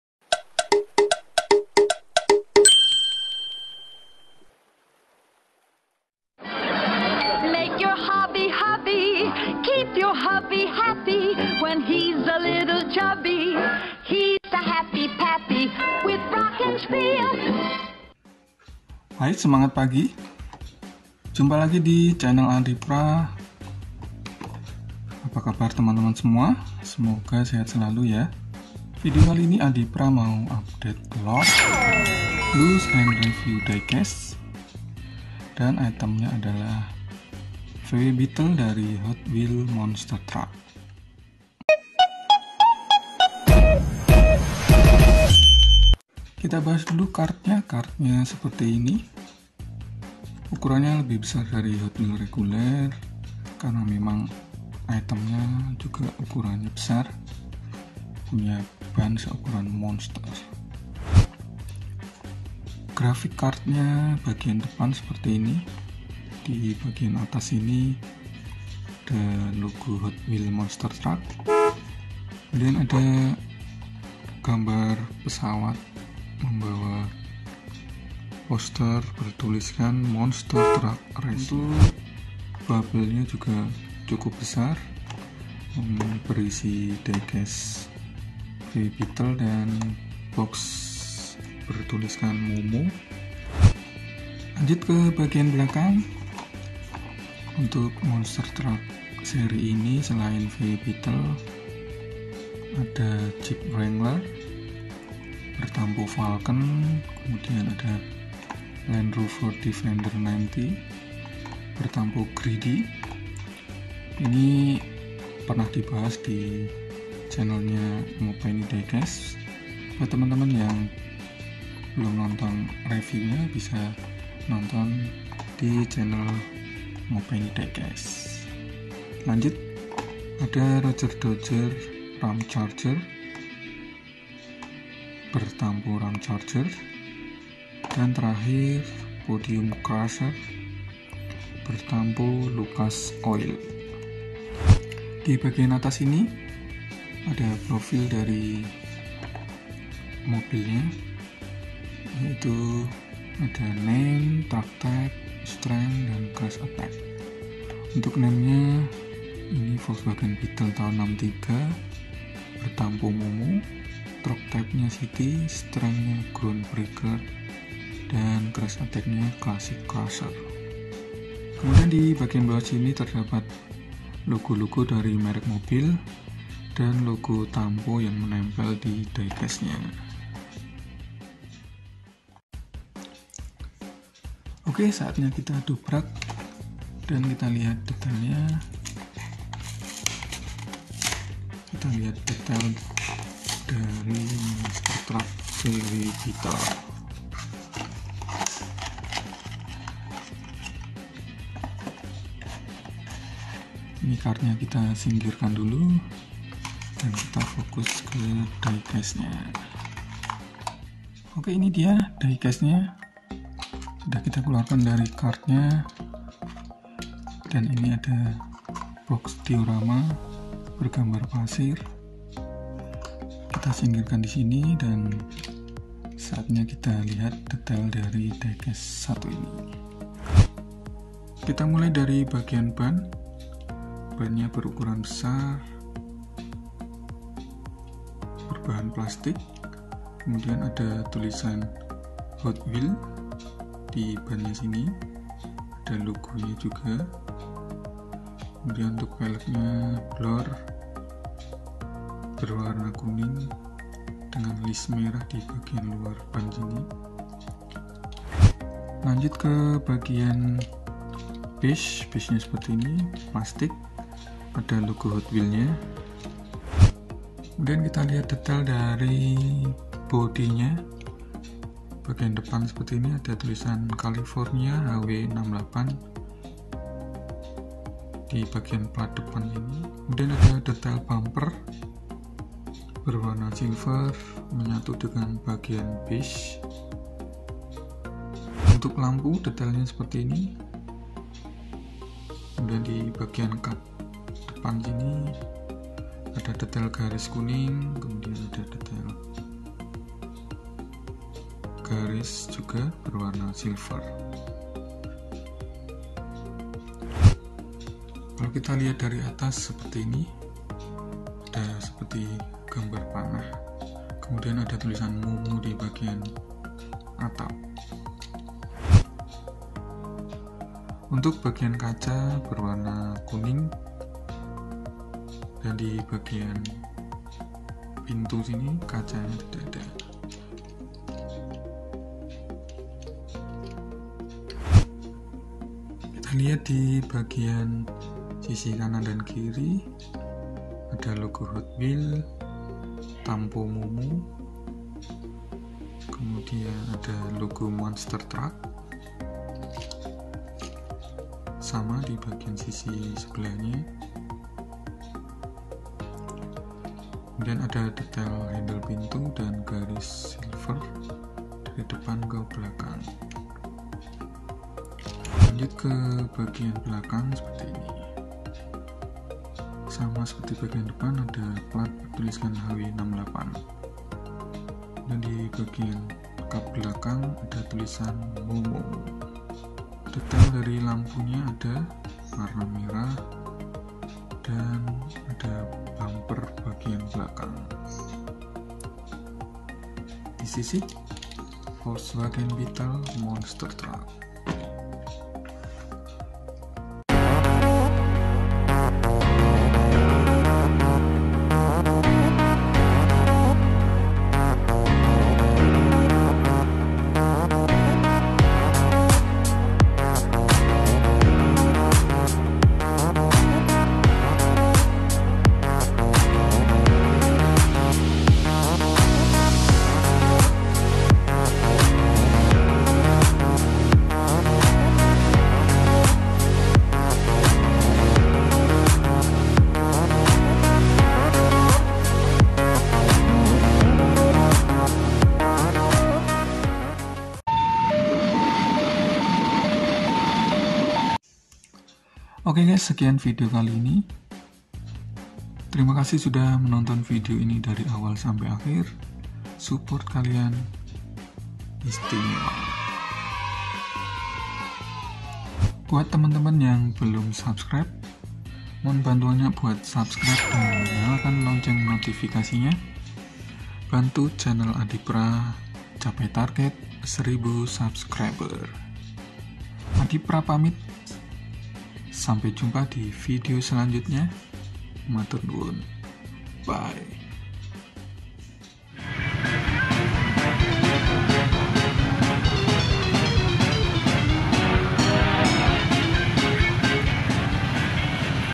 REVIEW DIECAST HOTWHEELS MONSTER TRUCKS VOLKSWAGEN BEETLE MOMO
Semangat pagi! Saatnya update fokusanadipraa, dan diecast yang akan kita bahas kali ini adalah HOTWHEELS MONSTER TRUCKS. Diecastnya adalah 1963 VOLKSWAGEN BEETLE MOMO. Yuk kepoin detailnya. maturnuwun.

Segmen on this video:
00:00 - Intro and Opening
00:46 - Review Card Monster Trucks VW Beelte Momo
03:02 - Dobrak Time
03:31 - The Details
06:37 - Showcase Volkswagen Beetle Momo
07:21 - Closing and Outro